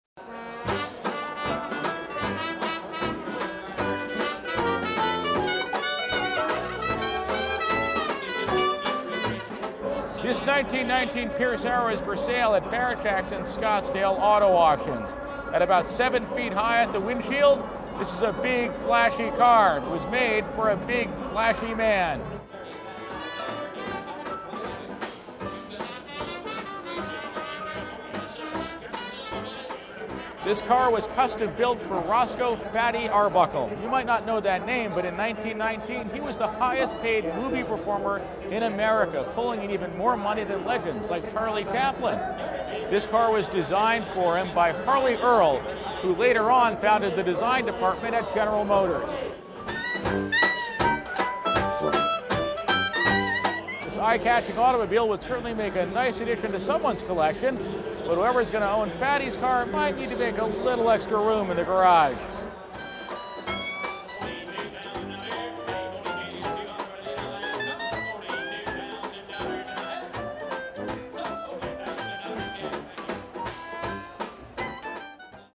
'Fatty' Arbuckle's big, purple, Pierce-Arrow
The custom built, 1919 Pierce Arrow was built for famous actor, Roscoe 'Fatty' Arbuckle, one of the highest paid folks in Hollywood, in the 1920s.